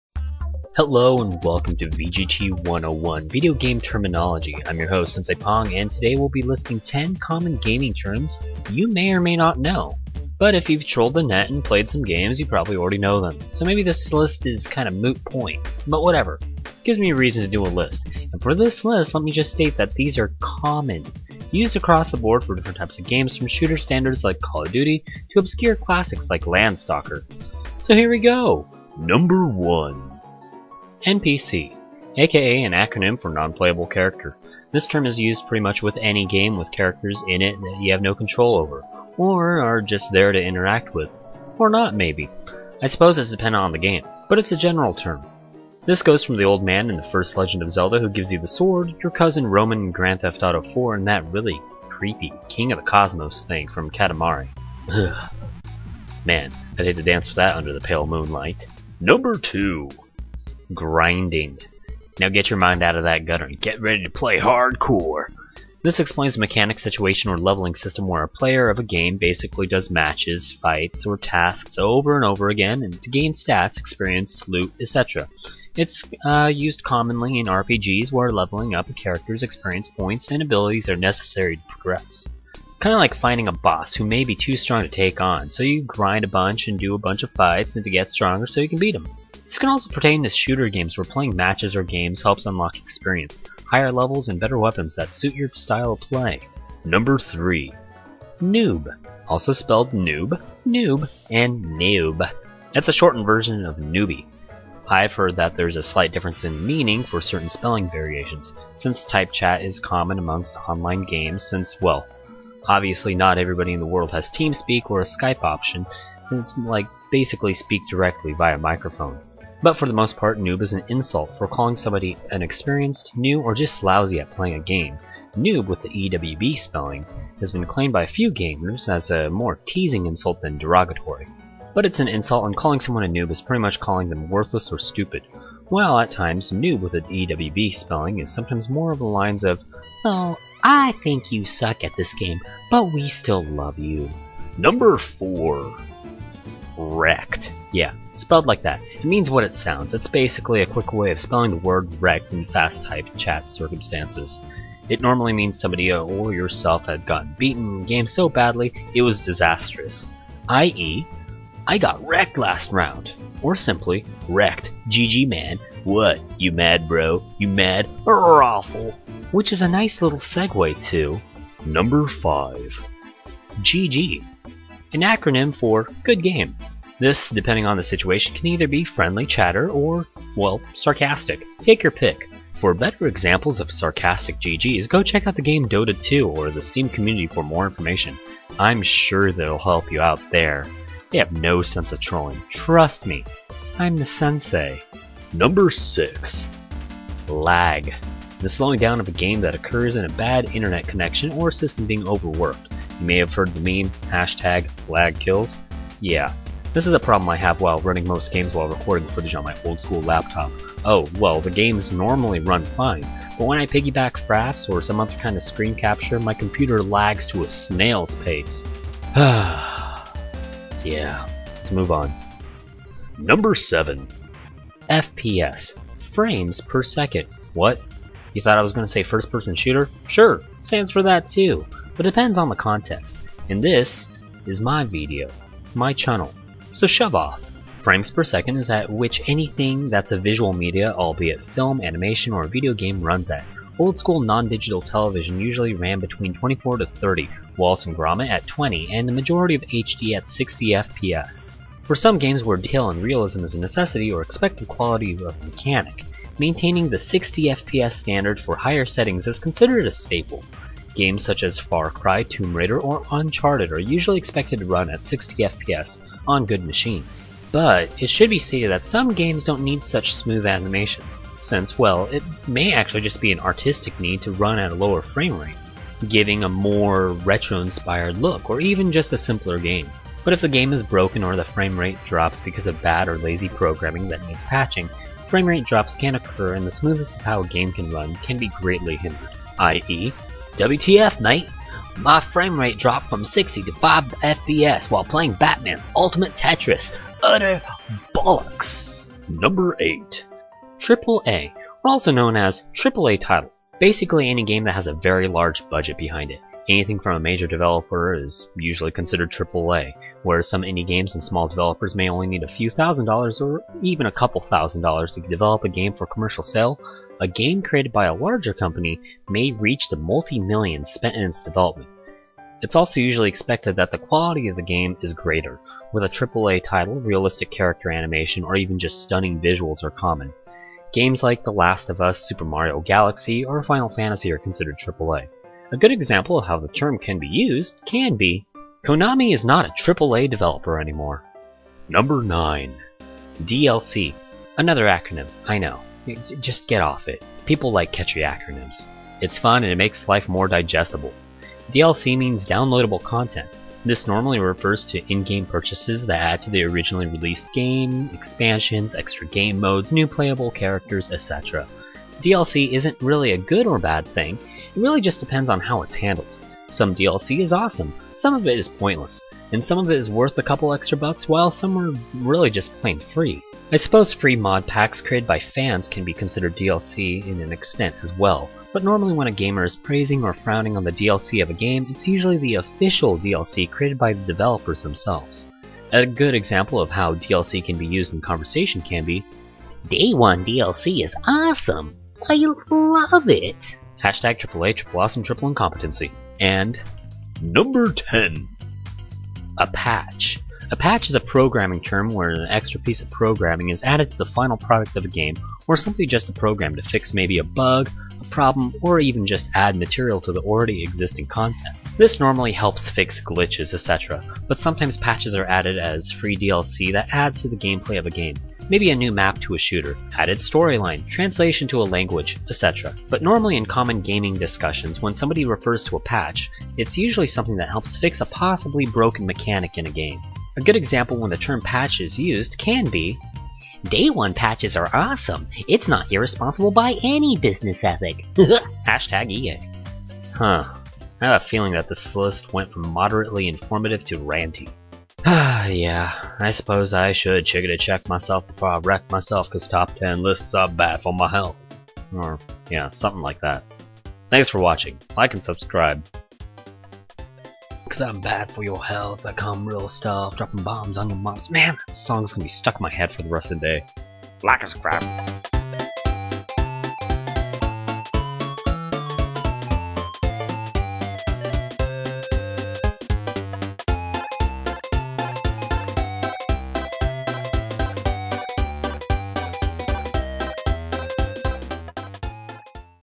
10 Common Gaming Terms and Slang - Video Game Terminology 101
I finally do a list of 10 things. That's one YouTube trope that can finally be checked off my list.

Lick and Scrubside!